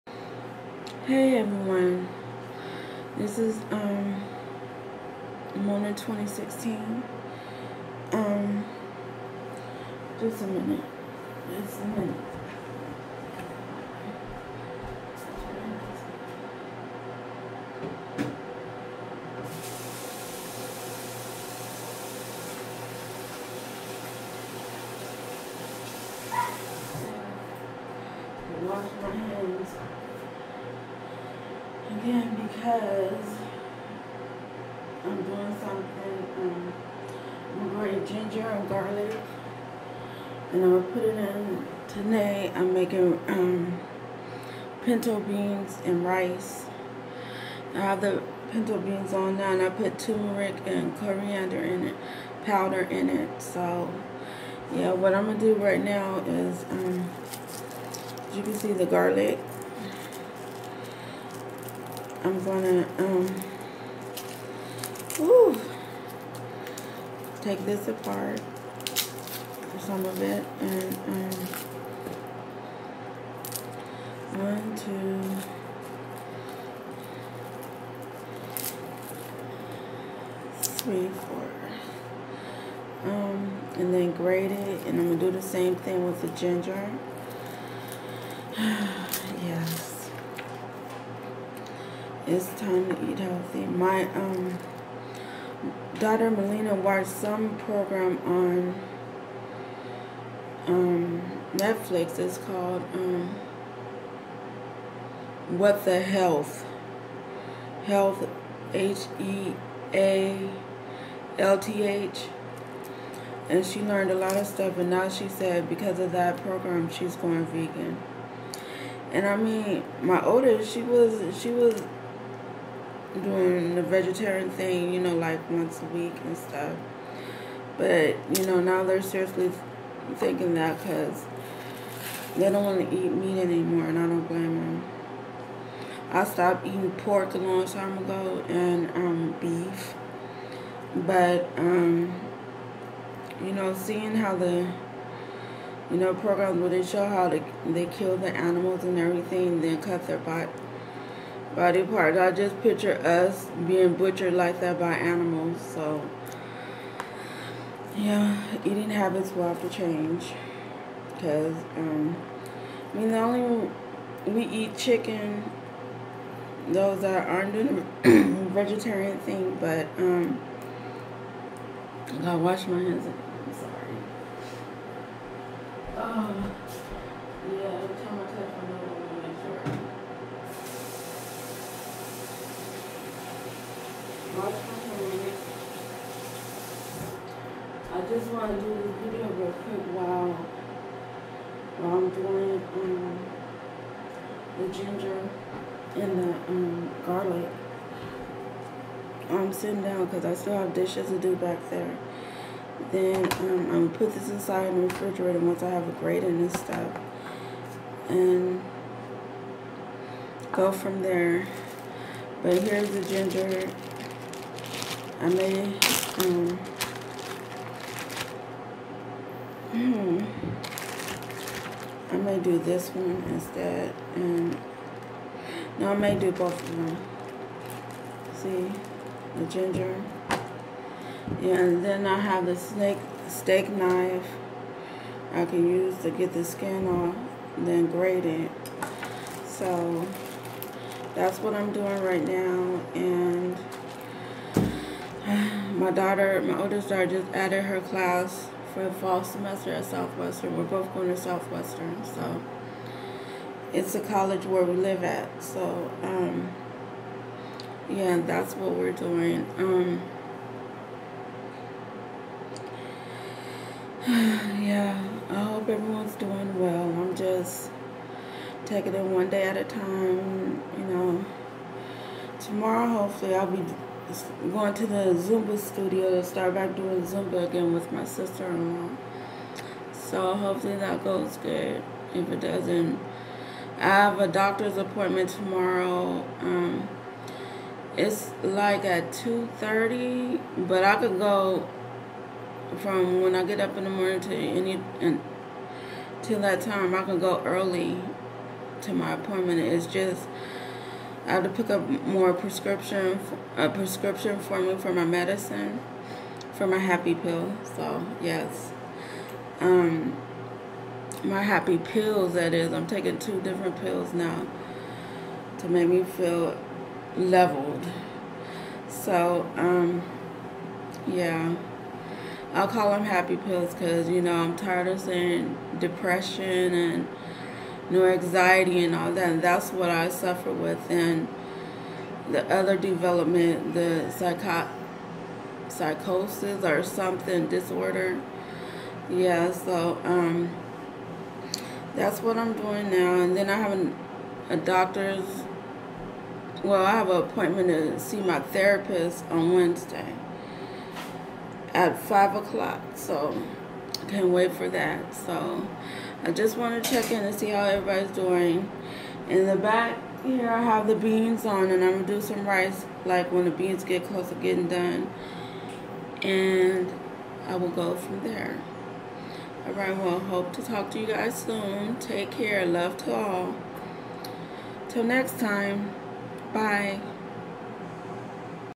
Preparing tonight's meal
Ginger and garlic paste for the pinto beans that I have on the stove cooking.